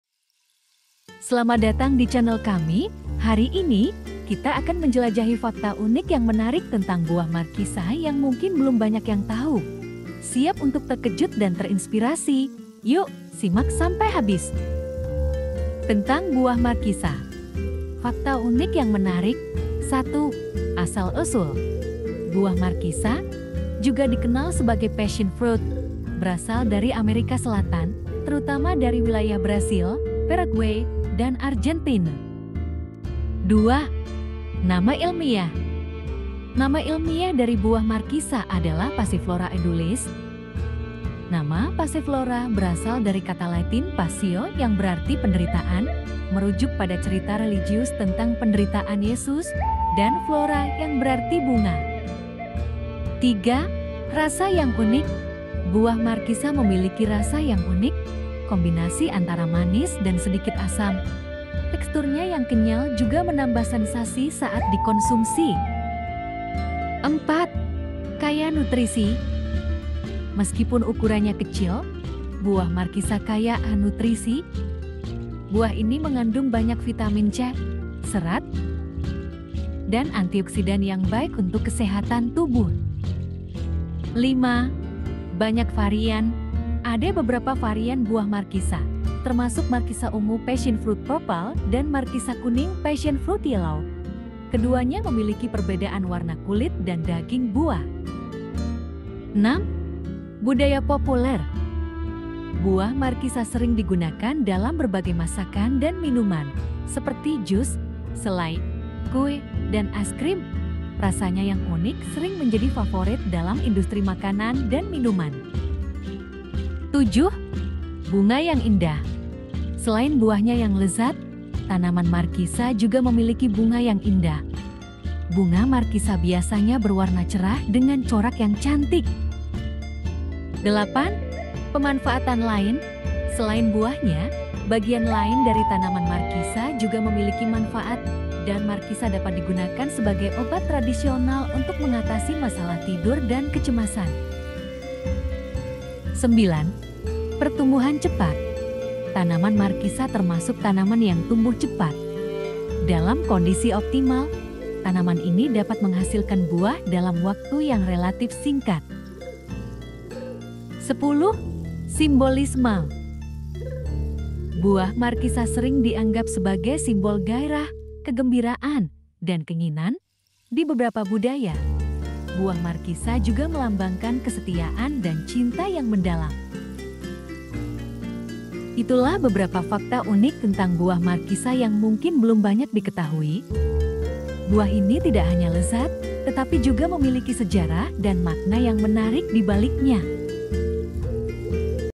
"Rahasia Tersembunyi Buah Markisa: Fakta Unik yang Menakjubkan!"
Selamat datang di channel kami! Siapakah yang menyangka bahwa buah markisa memiliki begitu banyak fakta unik dan menarik di baliknya? Dari asal usulnya yang eksotis hingga manfaat kesehatannya yang luar biasa, mari kita telusuri bersama keajaiban buah markisa dalam video ini! Jangan lewatkan kesempatan untuk menggali lebih dalam tentang buah yang penuh misteri ini.

Buah Markisa, Fakta Unik, YouTube, Rahasia Buah Markisa, Manfaat Kesehatan, Asal Usul Buah Markisa